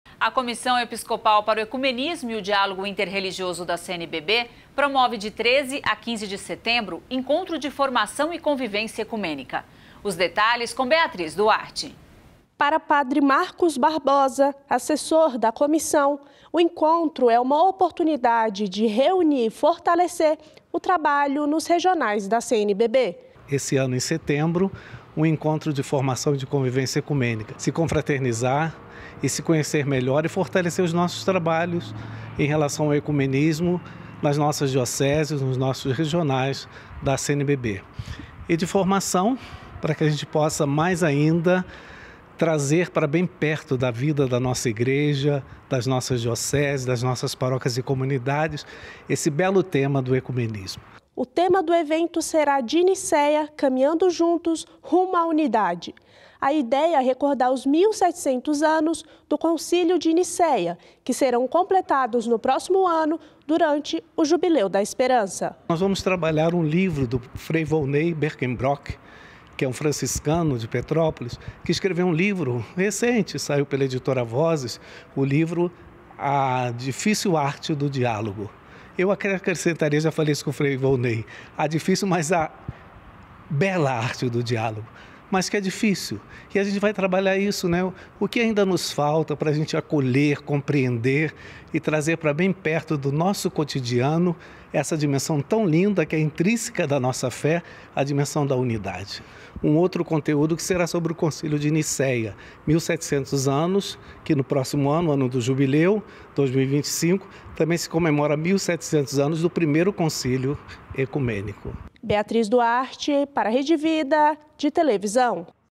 Encontro de Formação e Convivência Ecumênica é realizado pela CNBB – JCTV – 10/09/24
A Comissão Episcopal para o Ecumenismo e o Diálogo Inter-Religioso da CNBB promove, de 13 a 15 de setembro, Encontro de Formação e Convivência Ecumênica. Os detalhes com Beatriz Duarte.